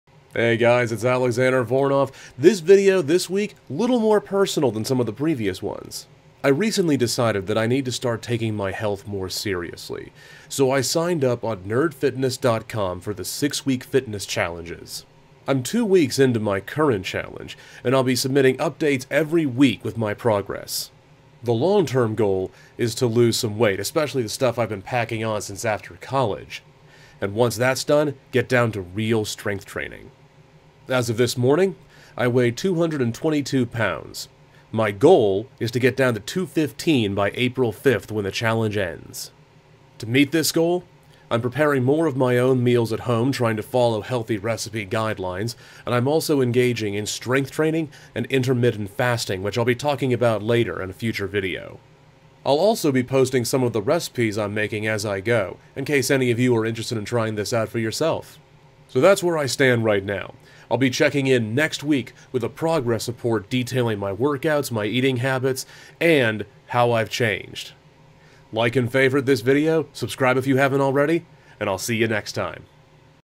Nerd Fitness: Challenge Accepted!
Trying to get healthy, I've committed myself to a new fitness challenge with Nerd Fitness.